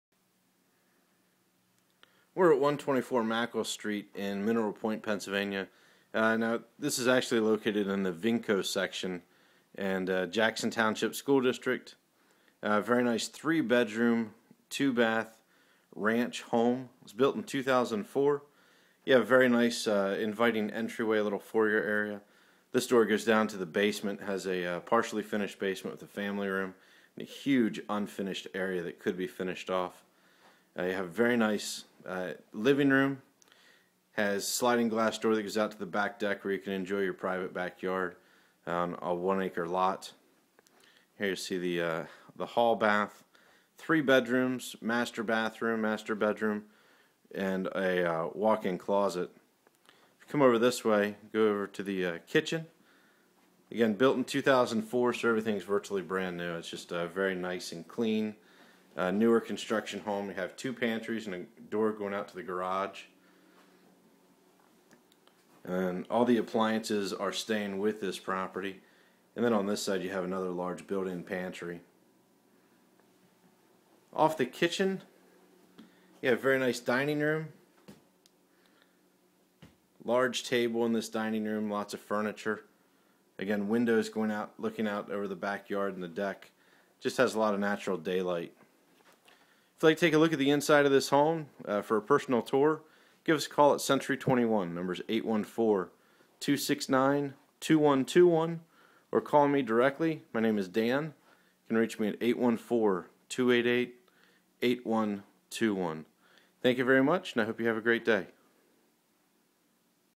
124 Mackall St Johnstown, PA
Located in Jackson Twp (Vinco Area), this ranch home was built in 2004 and includes 3 Bedrooms, 2 Bathrooms, large rooms, lower level Family Room, Walk-Out Basement on a large Park-Like lot on just over 1 Acre. Large Rear Deck with privacy.